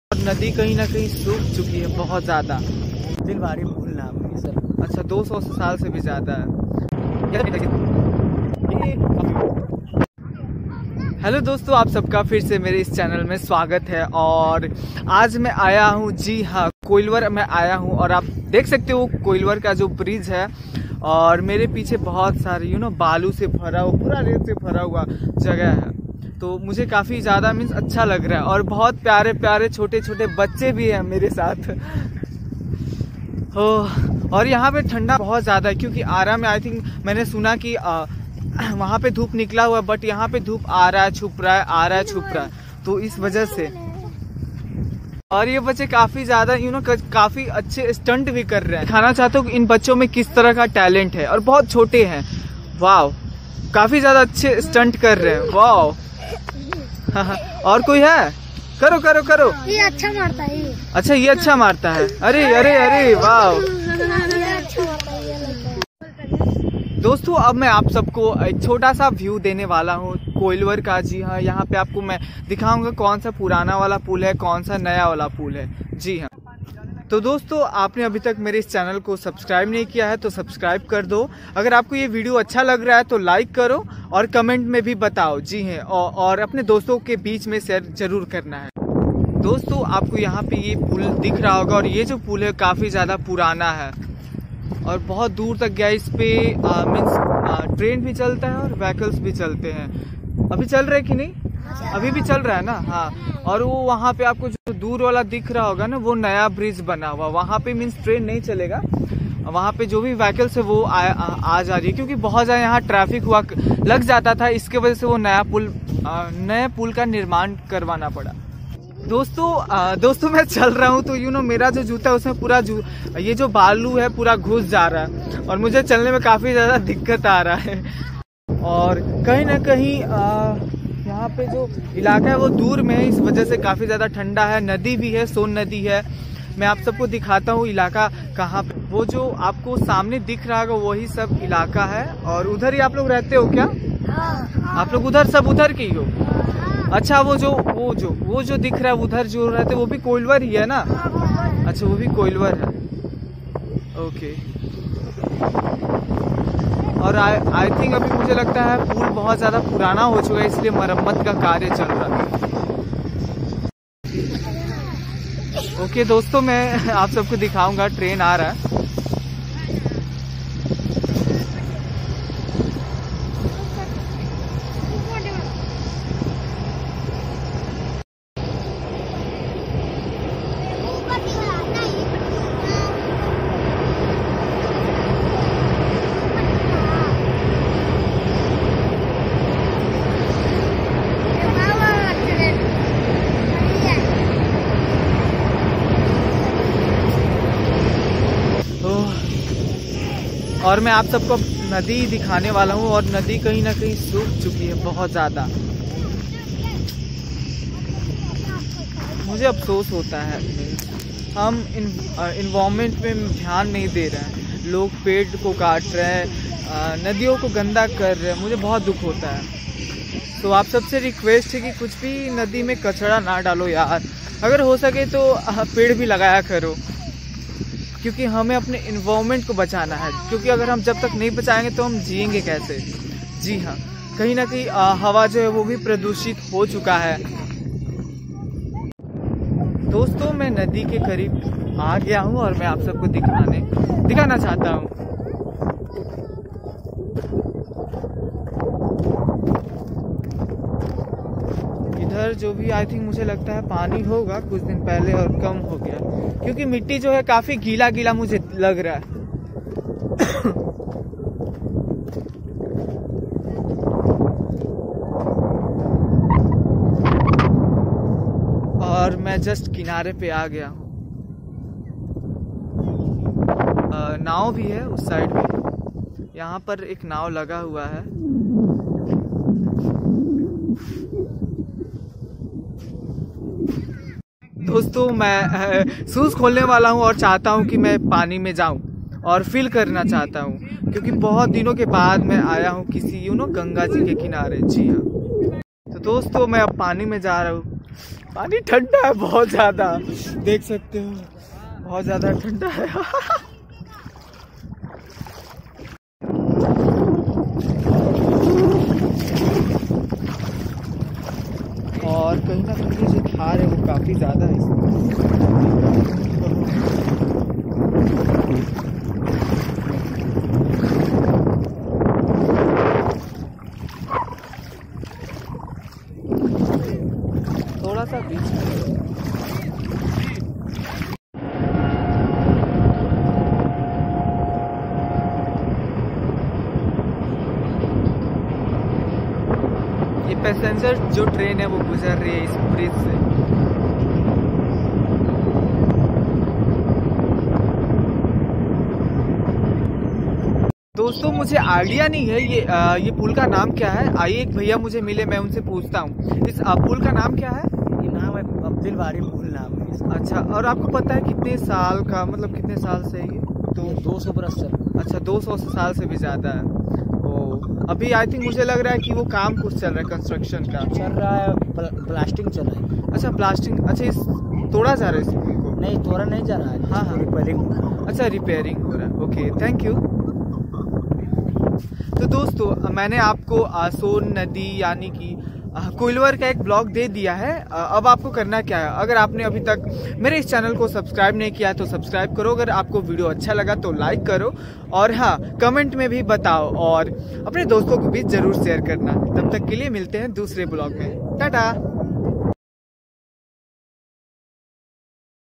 Shivam's Vlogs || Son Nadi || koilwar (bihar )ara
hlo dosto i am fine and u guys are also fine . I can understand now a days i am not uploading videos because of my phone . yes my phone is not working properly .  so in this video I was in koilwar so i have share a vlog on this particular place . and i know definitely u guys will enjoy so watch my full vlog and don't forget to subscribe , like , comment and share also 🙏🏻🙏🏻🙏🏻